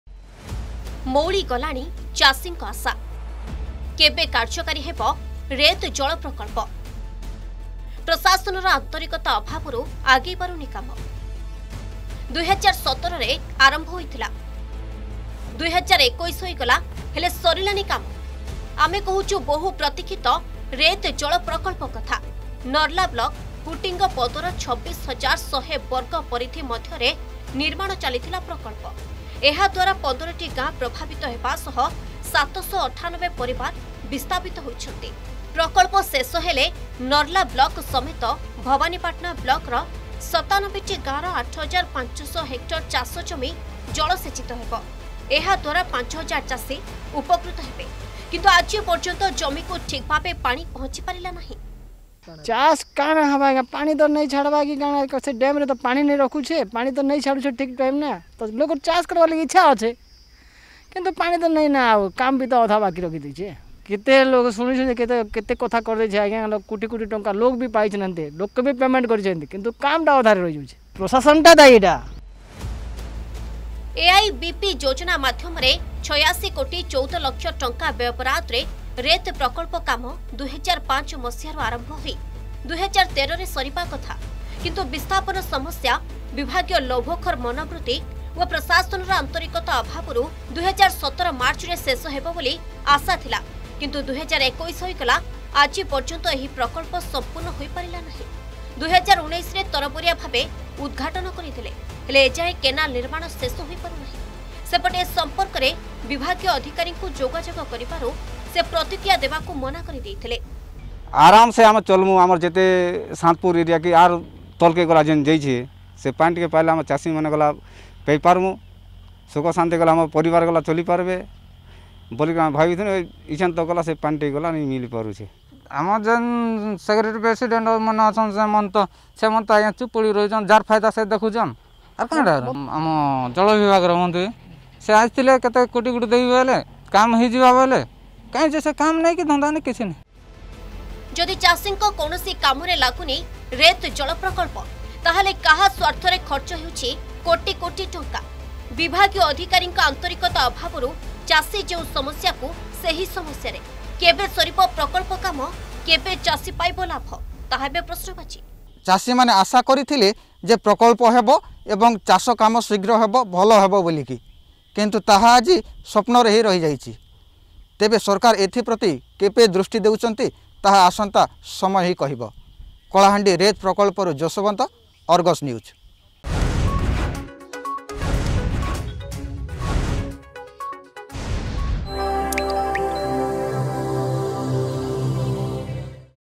ମଉଳି ଗଲାଣି ଚାଷୀଙ୍କ ଆଶା
କେବେ କାର୍ଯକାରୀ ହେବ ରେତ୍ ଜଳ ପ୍ରକଳ୍ପ ? ପ୍ରଶାସନର ଆନ୍ତରିକତା ଅଭାବରୁ ଆଗେଇ ପାରୁନି କାମ । 2017ରେ ଆରମ୍ଭ ହୋଇଥିଲା । 2021 ହୋଇଗଲା, ହେଲେ ସରିଲାନି କାମ